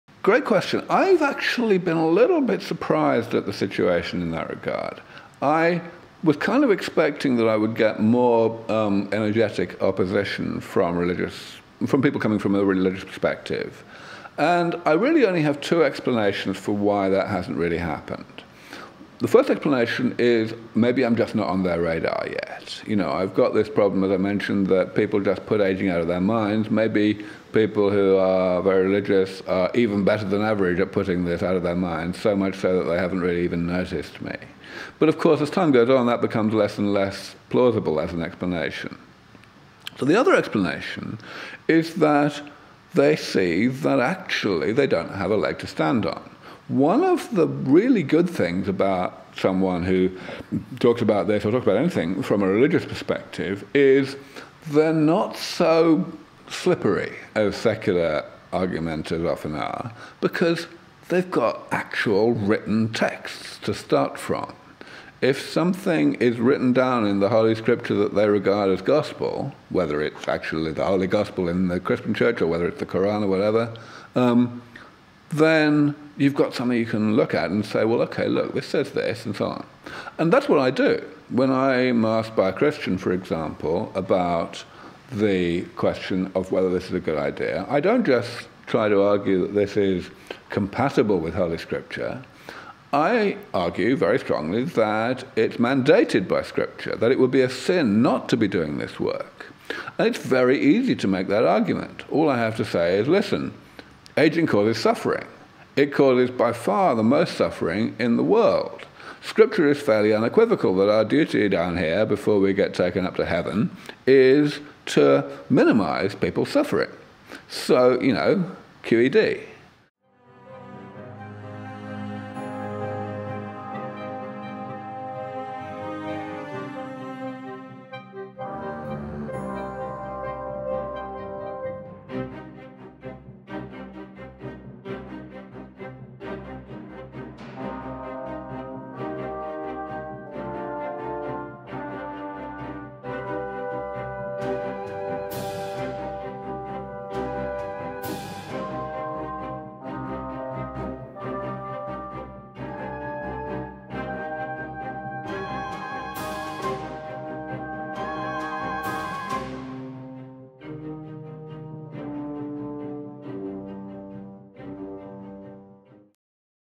How The Religious Texts Back The End of Ageing - Aubrey de Grey | London Real
Dr. Aubrey de Grey, Biomedical Gerontologist

Aubrey de Grey is a true frontiersman, daring to push out against what seems the most natural and unstoppable forces of nature - ageing. He’s not just another voice though, he’s a scientist and identifies ageing as a disease, one that can be cured with the right medicine. His work calls for serious scientific exploration of what causes tissue to age and to then find solutions to those components - what he calls the roadmap to defeat biological ageing. In fact, he believes that the first humans who will live to be 1,000 years old are already alive today!

Ageing, and indeed death, are some of the most colossal challenges to humanity biologically and philosophically. De Grey himself, of course, has caused plenty of controversy with his views. Despite, however, a $20,000 prize to disprove the worthiness of de Grey’s studies (put up by himself and MIT and de Grey’s own foundation) his work still stands, capturing the imagination of molecular biologists and the public alike.

We’re excited this week to welcome him to London Real. With his impressive beard and extensive knowledge on the subject we’re in for something special.